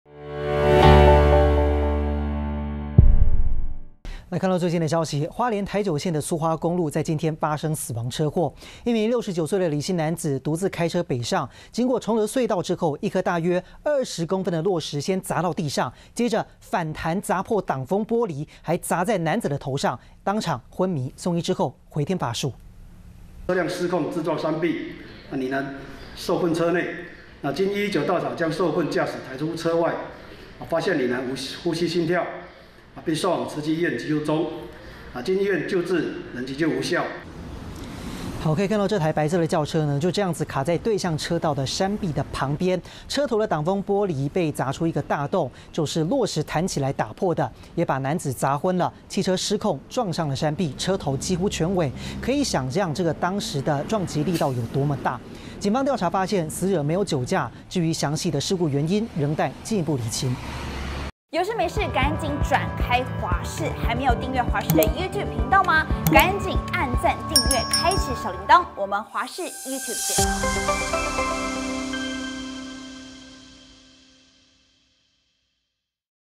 蘇花公路落石砸車 失控撞山壁駕駛不治｜華視新聞 20220224
花蓮台9線的蘇花公路上，今天發生死亡車禍，一名69歲的李姓男子，獨自開車北上，經過崇德隧道後，一顆大約20公分的落石先砸到地上，接著，反彈砸破擋風玻璃，還砸在男子的頭上，因此當場昏迷，送醫後，仍然回天乏術。